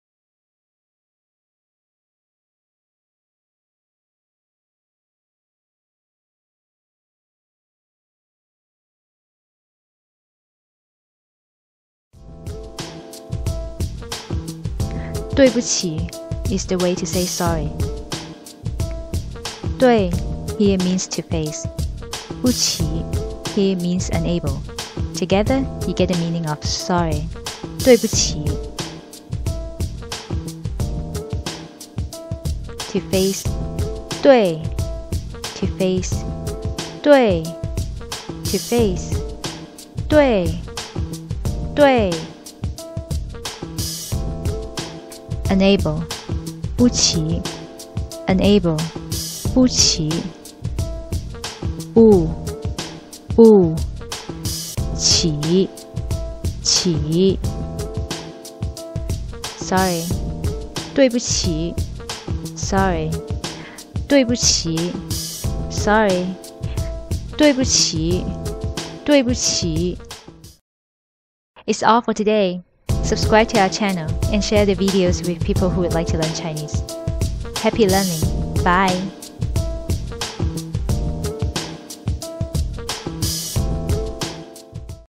Learn How To Say Sorry in Chinese
Sorry
对不起 - duì bú qì
In this video lesson we will teach you how to say I'm sorry in Chinese.  Saying I am sorry is the best way to respond to a Chinese person who is upset with you, even if you're not sure what they're saying or what you did wrong.  It literally means "I cannot face you" because you are embarrassed or don't feel worthy.

Sorry!
对不起 - duì bù qǐ

How to Say I'm Sorry in Chinese
How to Say I am Sorry in Chinese